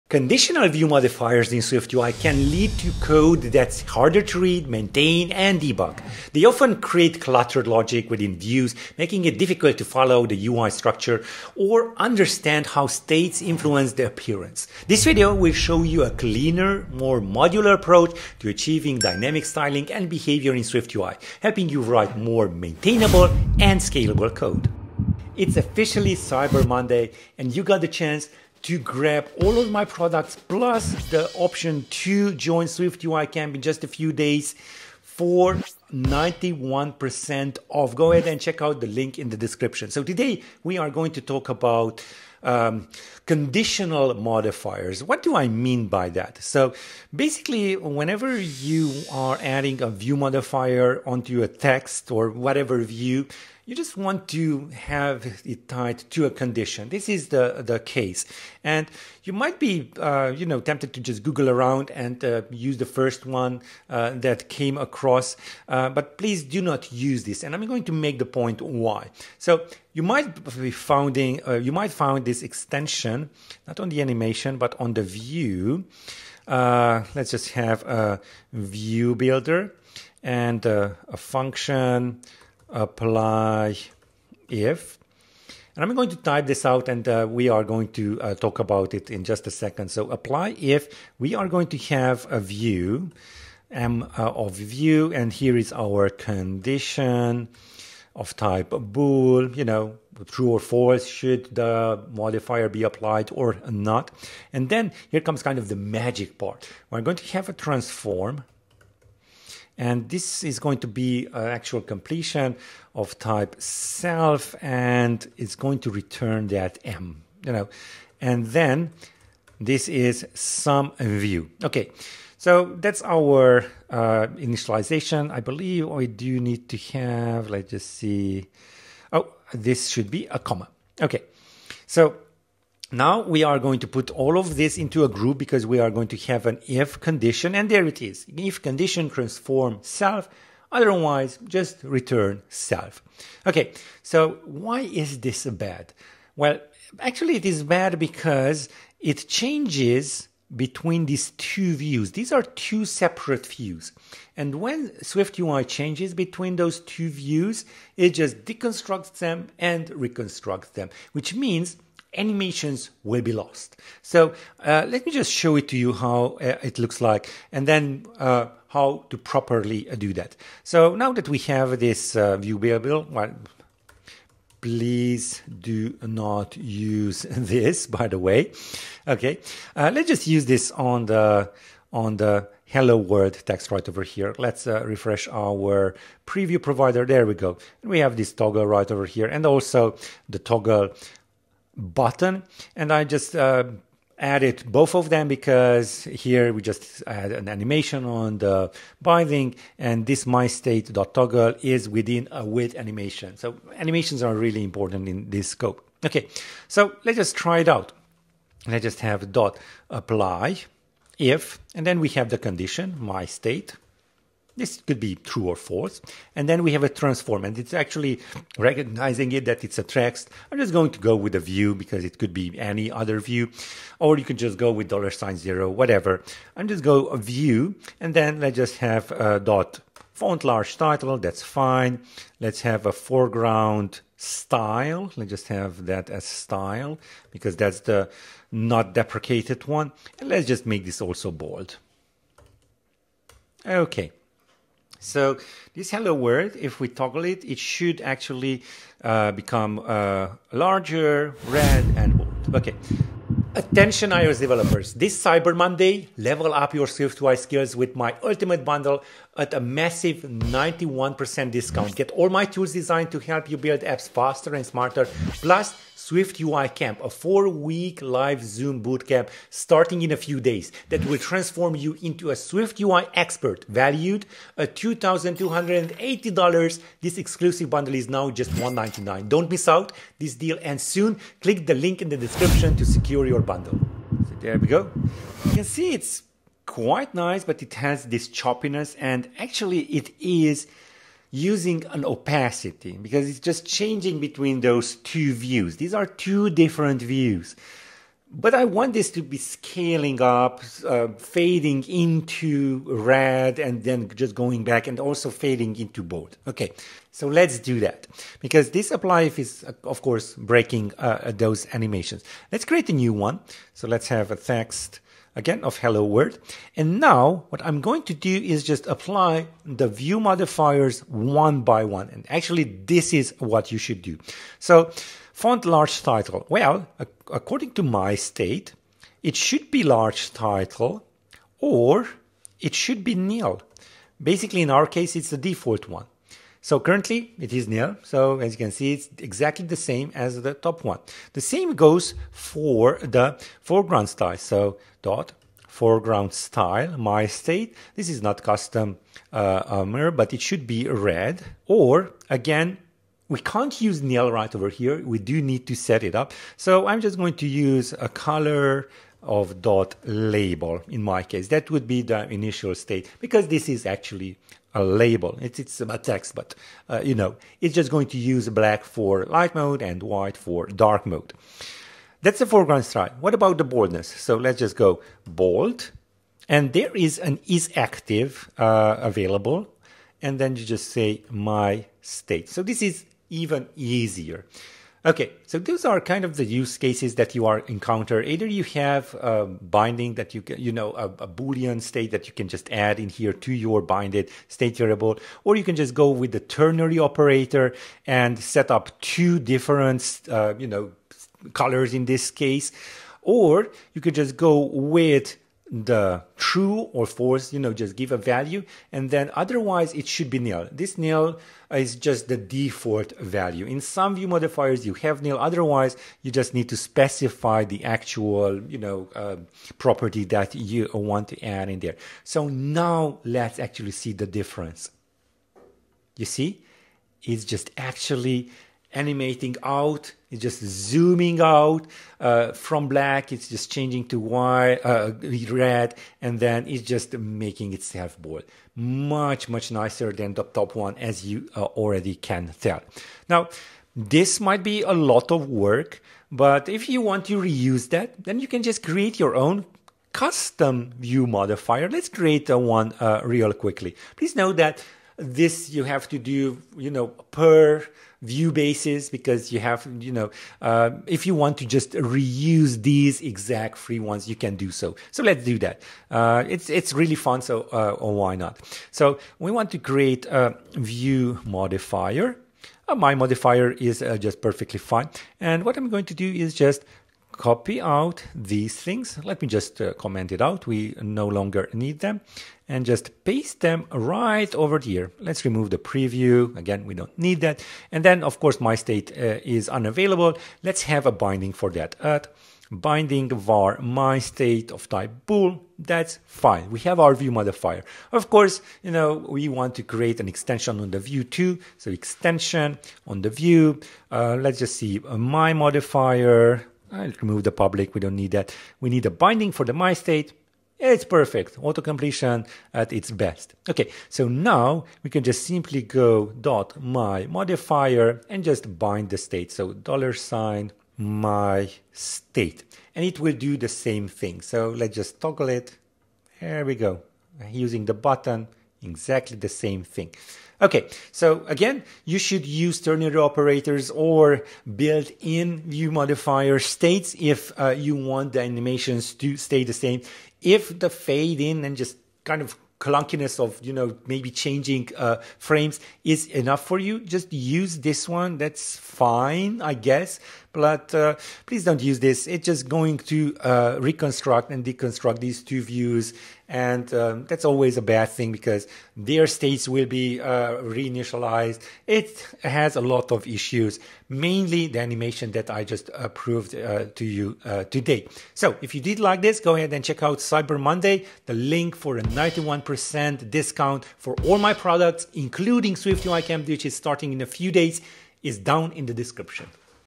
STOP Using Conditional Modifiers in SwiftUI! Here's What to Do Instead 🚀
Conditional view modifiers in SwiftUI can lead to code that's harder to read, maintain, and debug. They often create cluttered logic within views, making it difficult to follow the UI structure or understand how states influence the appearance.

This video will show you a cleaner, more modular approach to achieving dynamic styling and behavior in SwiftUI, helping you write more maintainable and scalable code. Say goodbye to messy conditionals and learn the right tools to simplify your SwiftUI views!

DO YOU WANT TO ME TO WORK ON YOUR PROJECT?